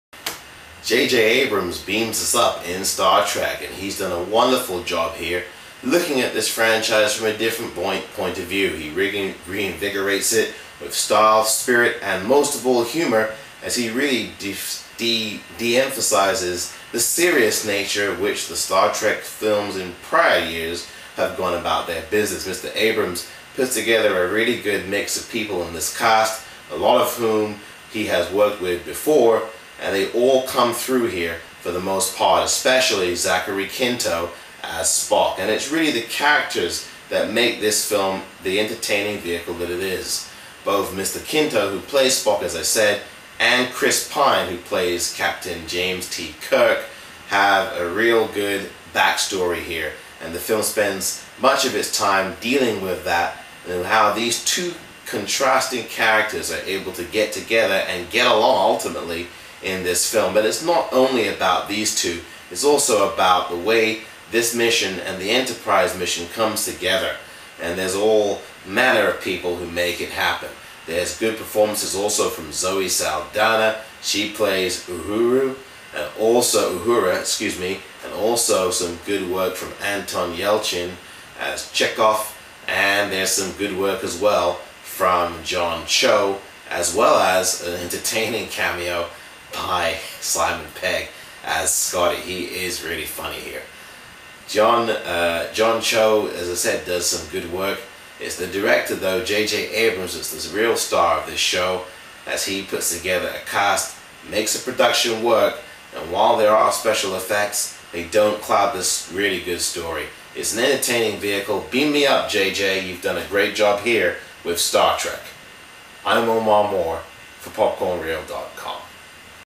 Star Trek Movie Review / PopcornReel.com
Omar P.L. Moore's unscripted review of "Star Trek", directed by J.J. Abrams.